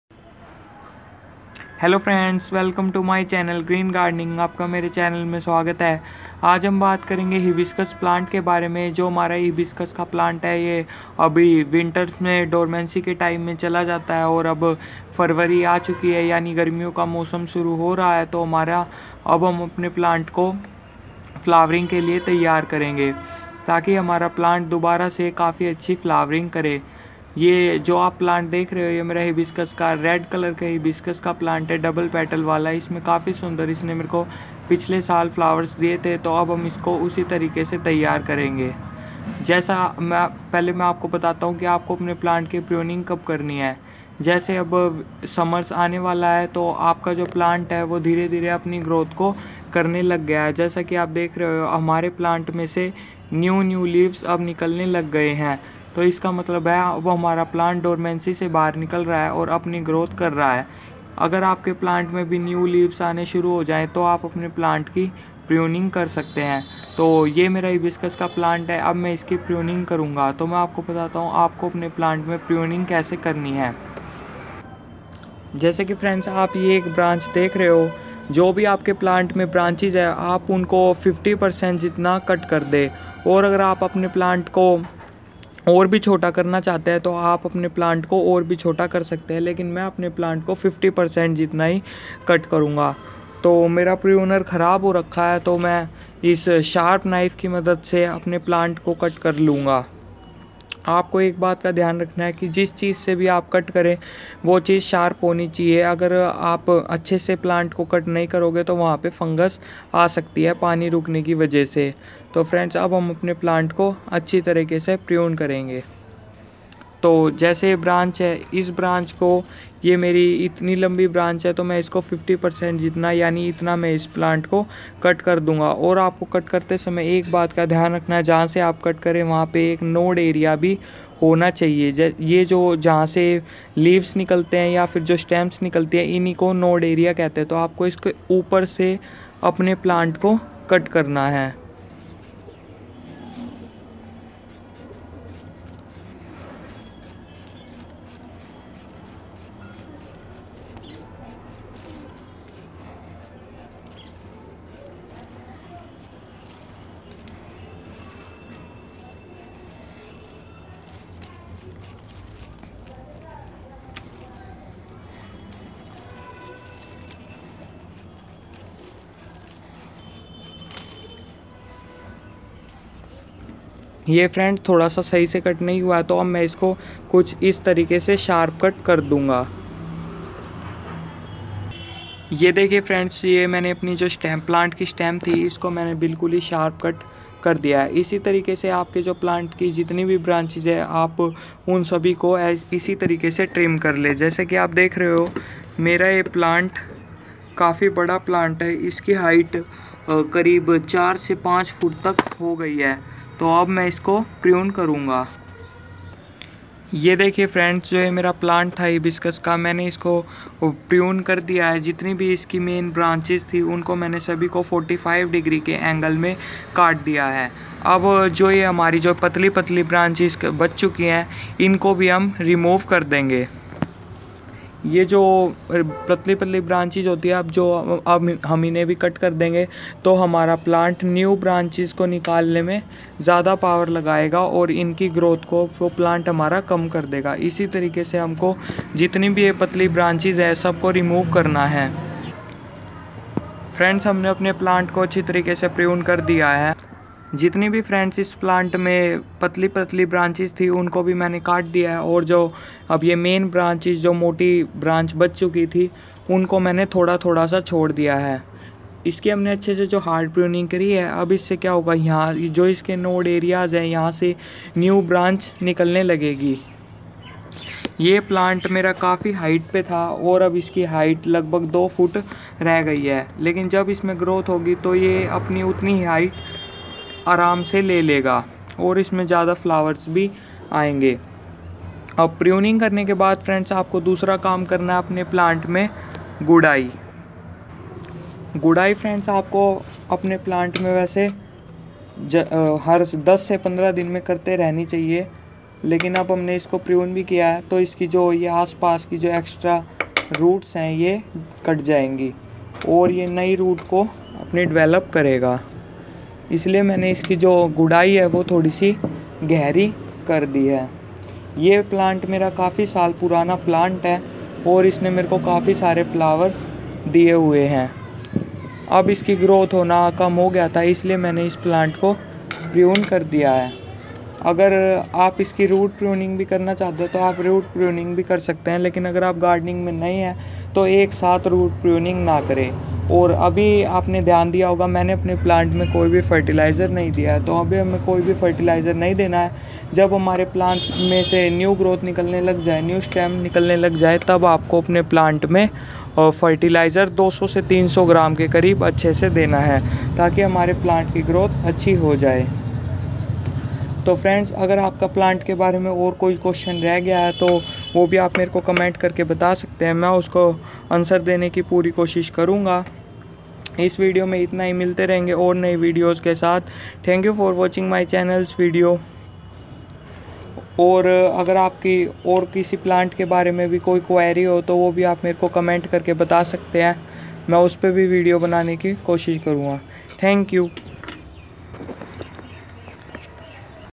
गुड़हल पर ज्यादा फुल पाने के लिए फरवरी मे करे ये काम🌺/Hibiscus plant pruning(cutting)in February
Hello friends 🙏🙏

This video about hibiscus plant :-

Hibiscus plant is very beautiful plant and very easy to care. this plant dormancy time is winter and groth time is summer.

So in summer in February plant need pruning for better groth. In this video we prune Hibiscus plant.
please watching full video.

Hibiscus plant care in summer

hibiscus plant pruning

hibiscus plant cutting

hibiscus plant fertilizer

If or any query sent by comment I try to give answers.

thankyou for watching my channel's video

Happy gardening 😀